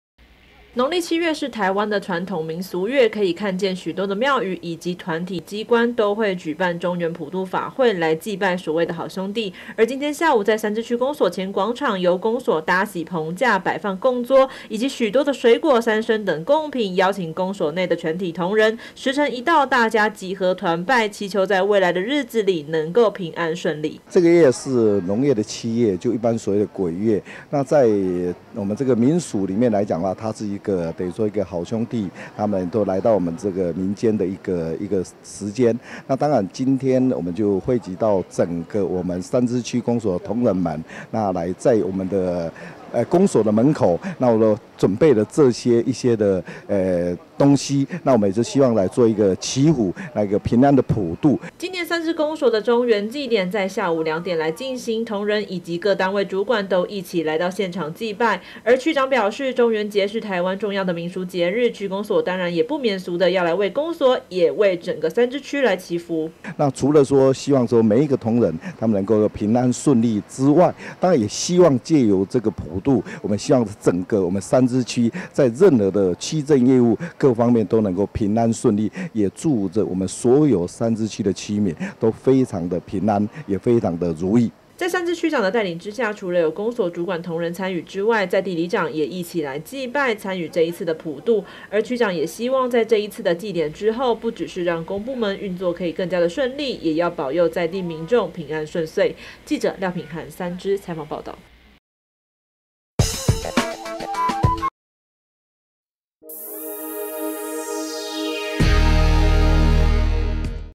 1050816 民俗月公所辦普渡 祈福地方平安順遂
今天下午，在三芝區公所前方廣場，由公所搭起棚架，擺放供桌，以及許多的水果、三牲等供品，邀請公所內的全體同仁，時程一到，大家集合團拜，區長希望在這一次的祭典之後，不只是讓公部門運作可以更加順利，也要保佑在地居民平安順遂。